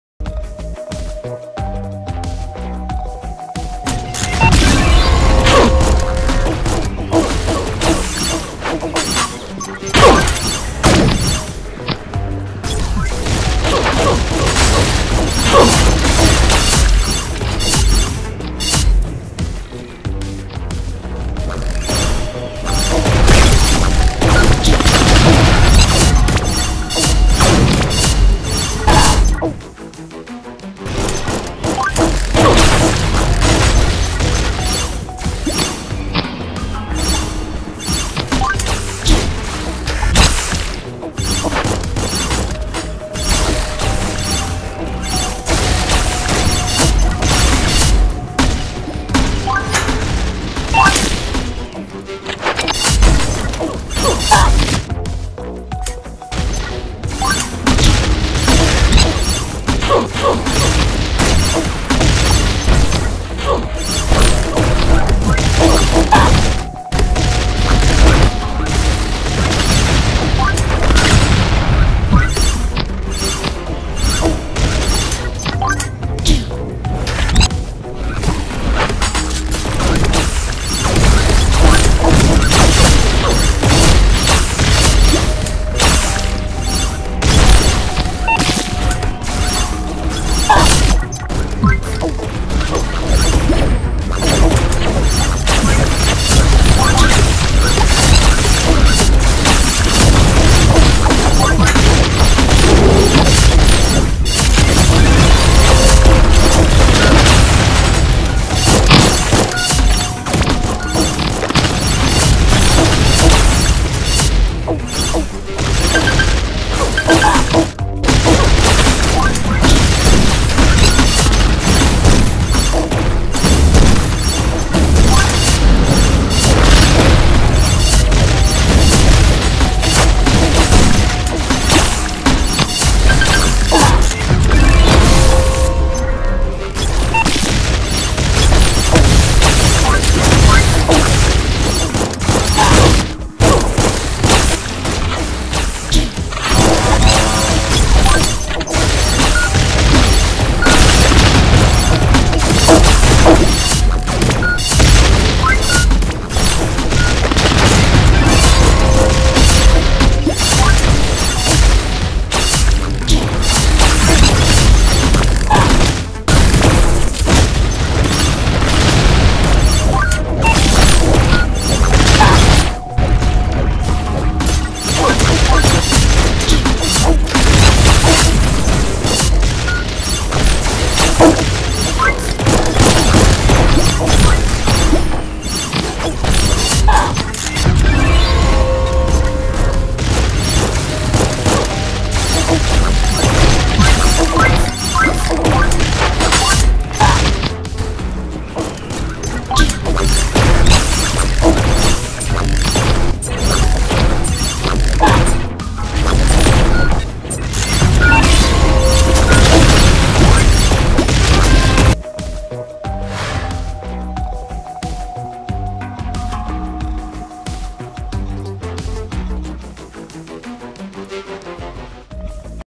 Pixel Stream Sword Gameplay
One of my favorite melees for combat, it does have a bit of pullout delay but it has the 85 mobility, a pretty decent attack speed, and 2 shots. A critical hit or headshot can 1 hit after the poison damage.